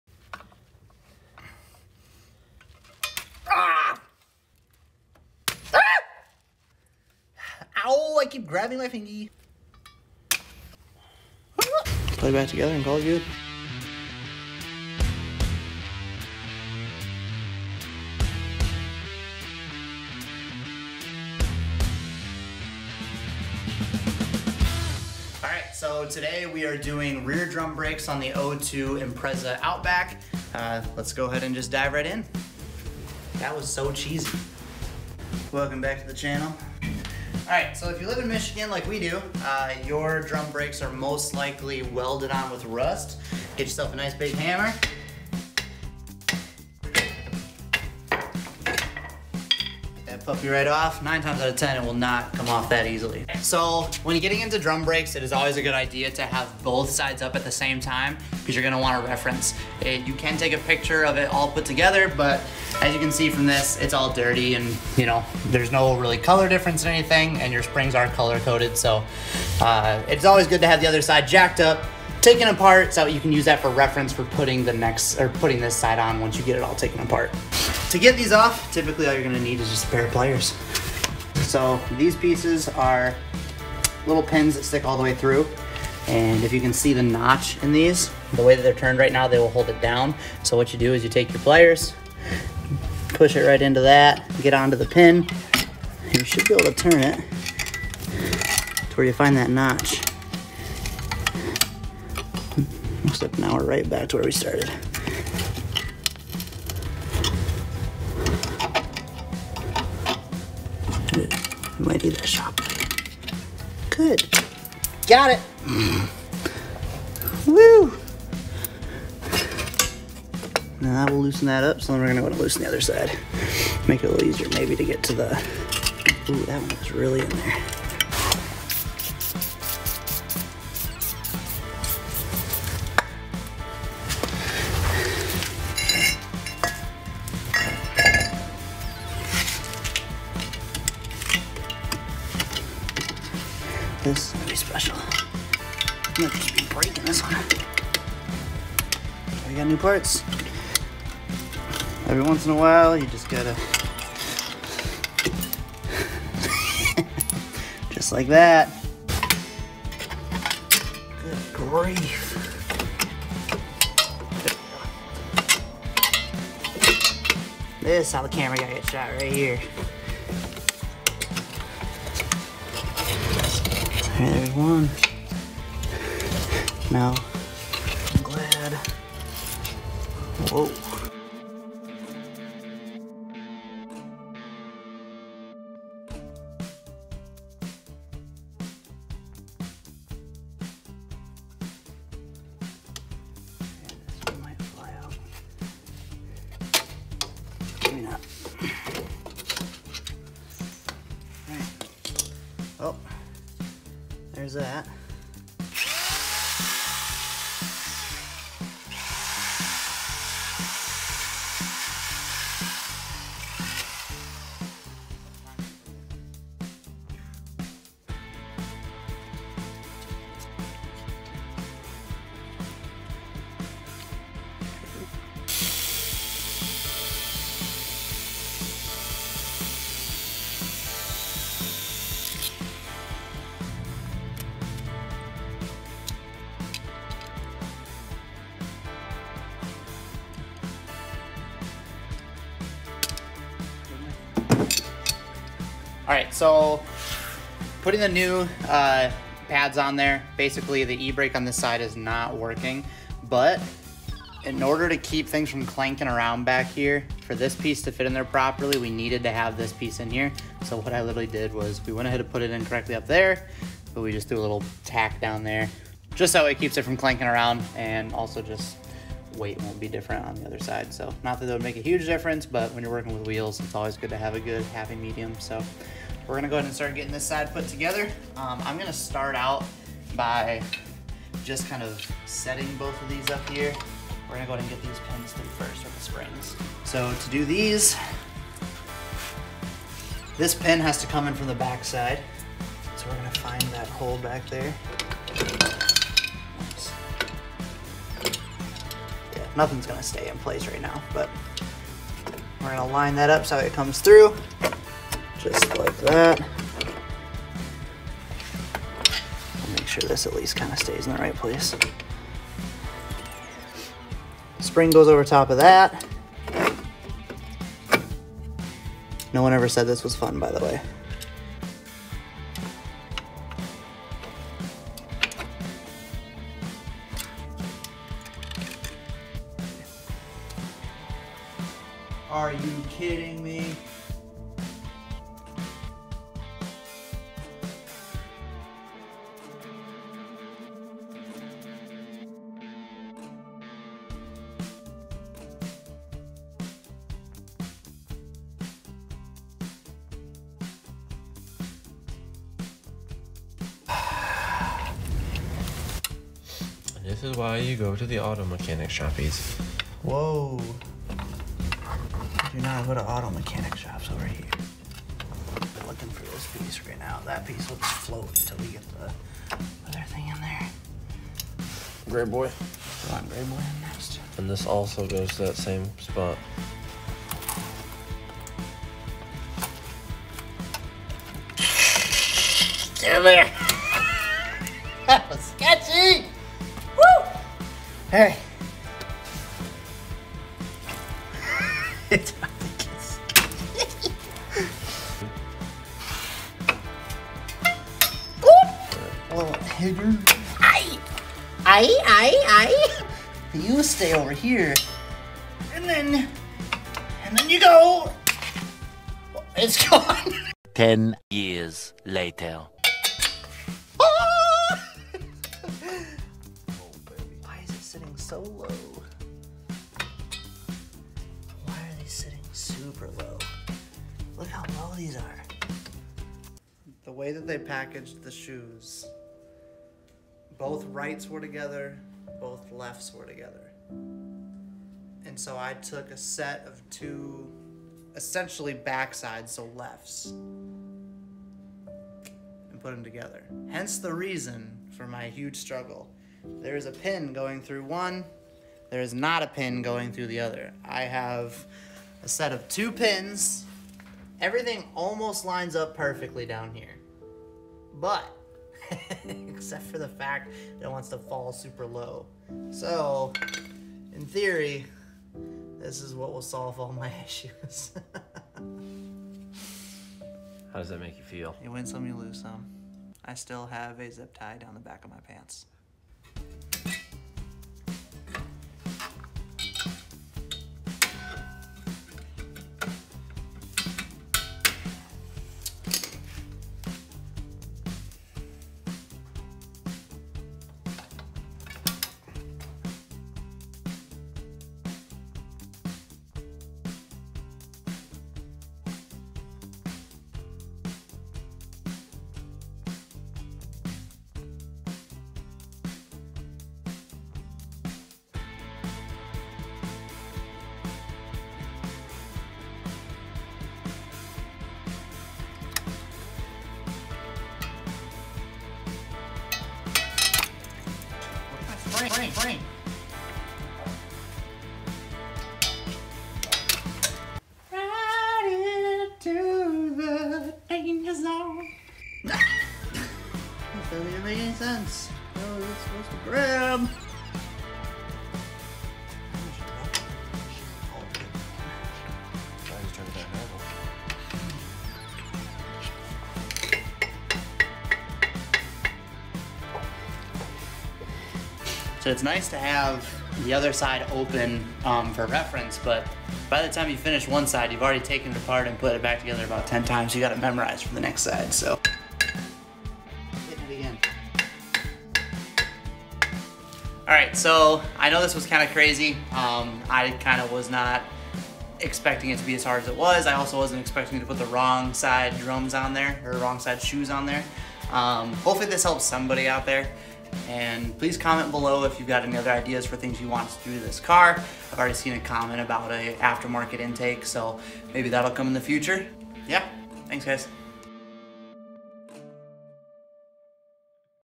Donuts Ep: 6 - 2002 Subaru Impreza Outback Drum Brakes
Watch today as Jared changes the rear drum brakes on his Subaru. Struggles and all, we hope this helps you get through your brake project and have a few laughs too.